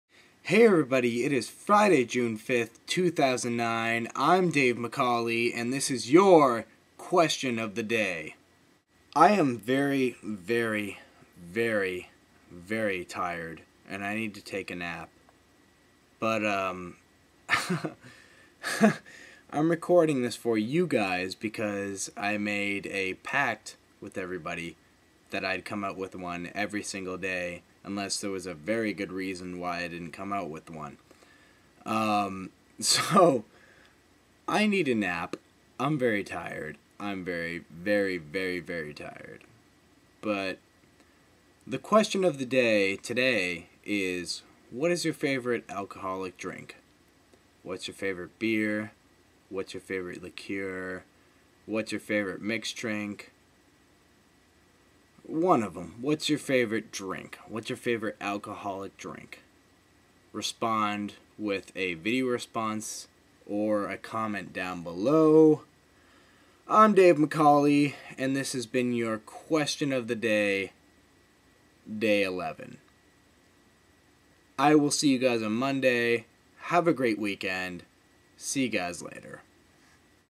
QOTD - Day 11
So, I recorded this on Friday morning but never got the chance to upload it. Here it is now though, on Sunday afternoon. It was the only question I had on my mind and I don't think I will get many answers because of the fact that I think a lot of the people that do this are under the age of 21 (USA) or 19 (Canada).

Yes, I was REALLY tired!